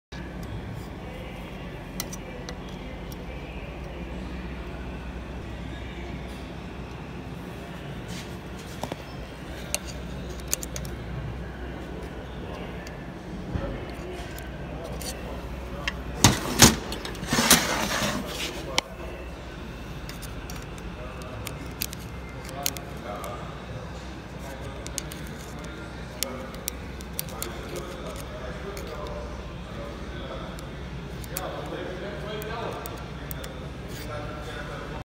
Turbo problemen
Turbo van Polo 1.6 tdi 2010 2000 km nieuw gemonteerd